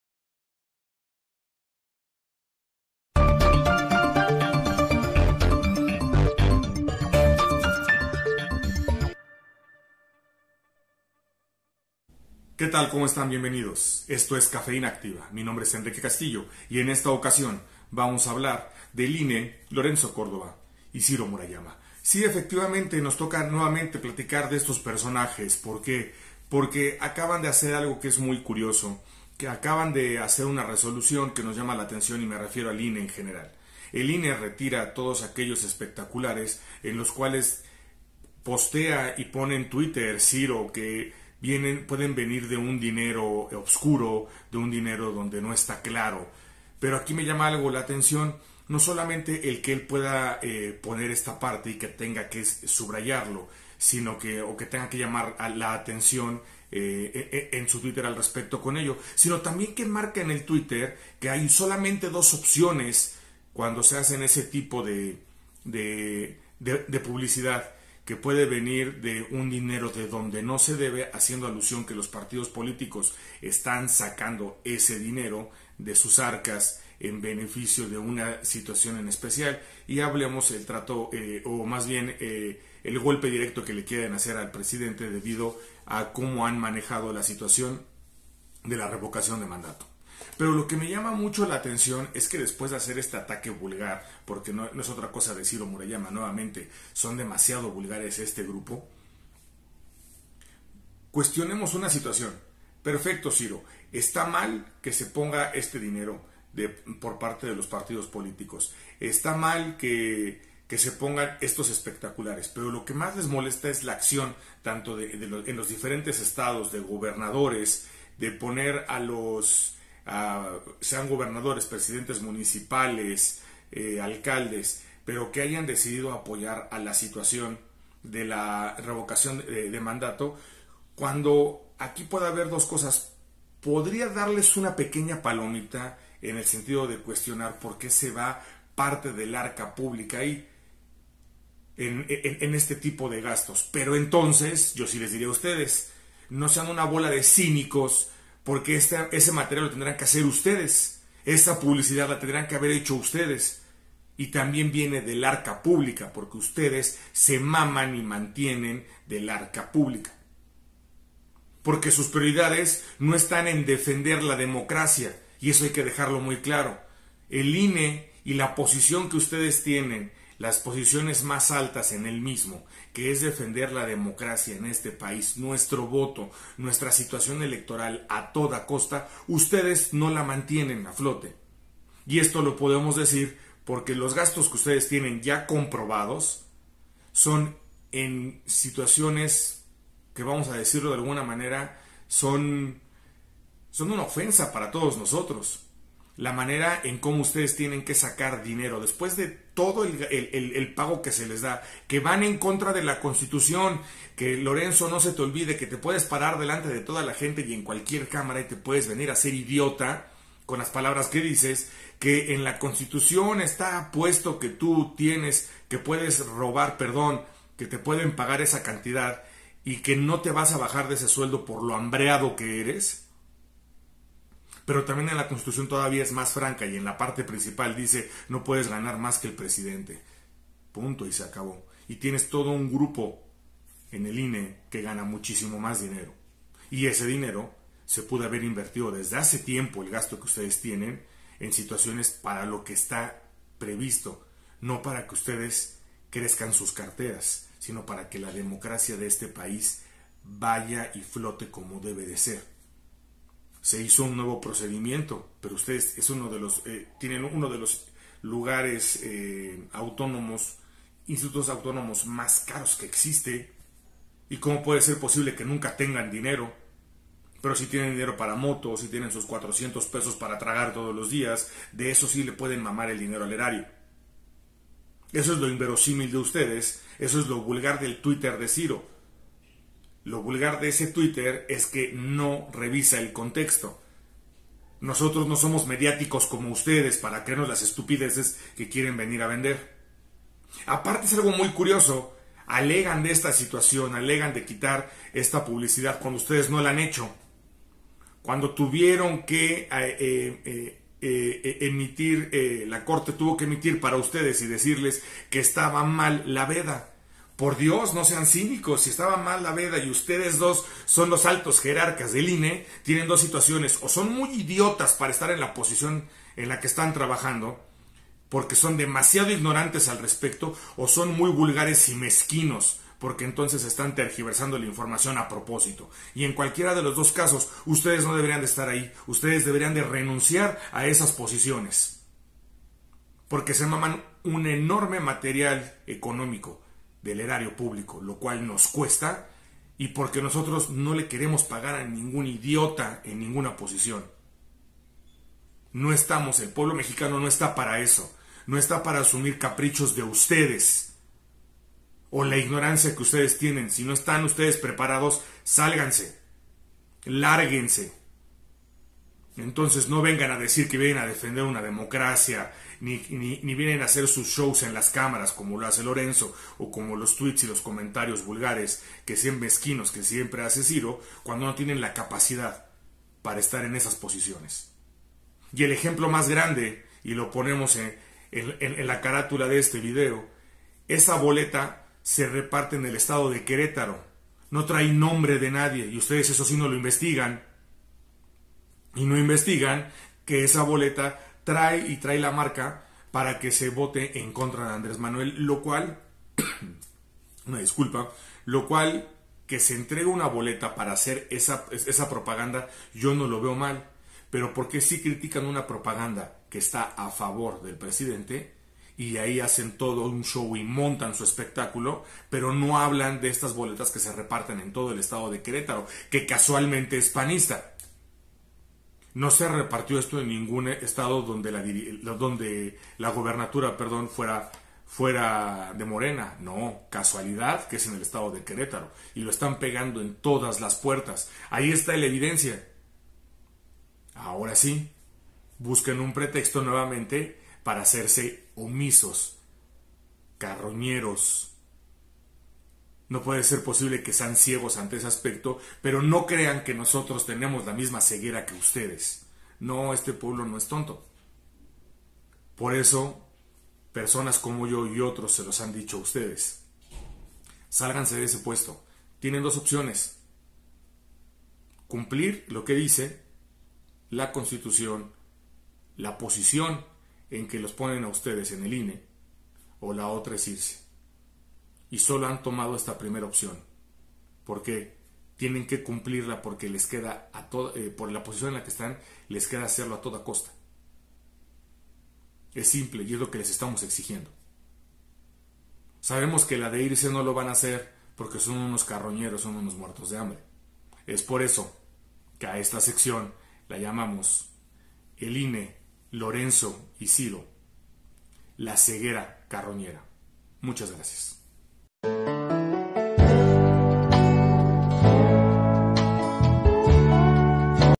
DEMOSTRACIONES: LORENZO Y CIRO. Ceguera Carroñera.
En este capítulo hablamos de la tendencia del INE para tomar decisiones.

No actúa de la misma manera en casos que resultan similares.

¿Porqué no investiga los diferentes casos de la misma manera?

¡Bienvenidos a... CAFEINA ACTIVA!